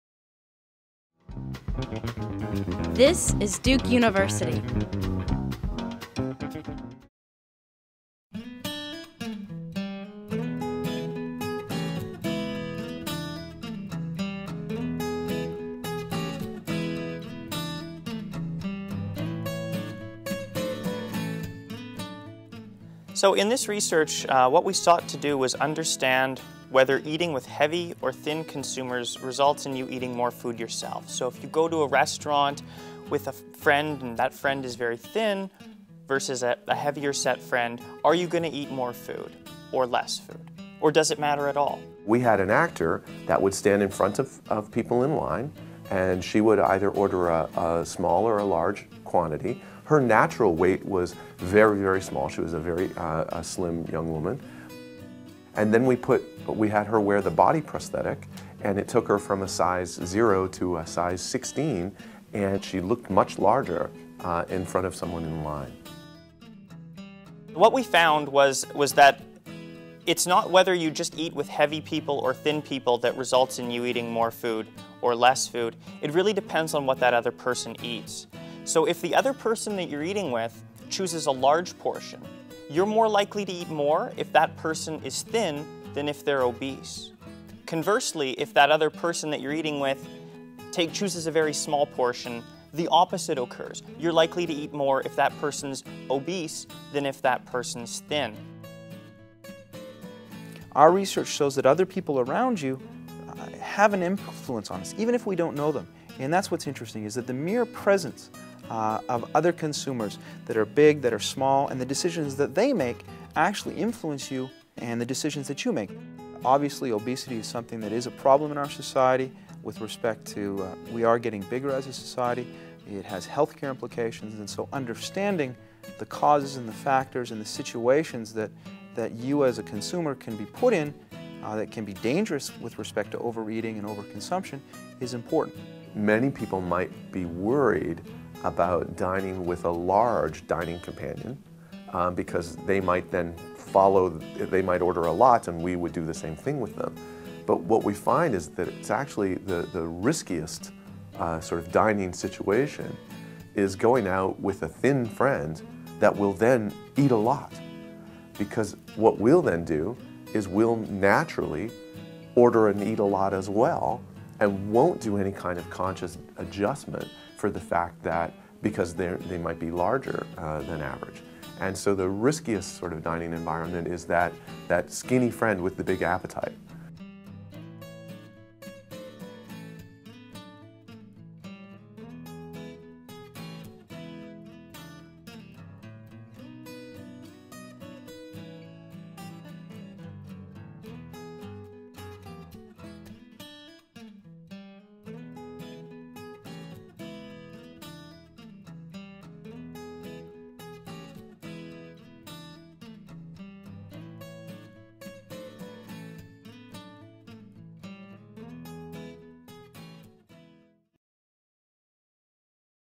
The Company You Keep Influences How Much You Eat
Thin friends who eat a lot could put your waistline in danger. That's the warning from researchers studying how other people's weight and food choices influence how much we eat.

Researchers from Duke University, the University of British Columbia and Arizona State University used snack foods, an obesity prosthesis, and the ruse of a study related to movies to track how students' food consumption was influenced by a companion. Their findings will appear online this week in the Journal of Consumer Research.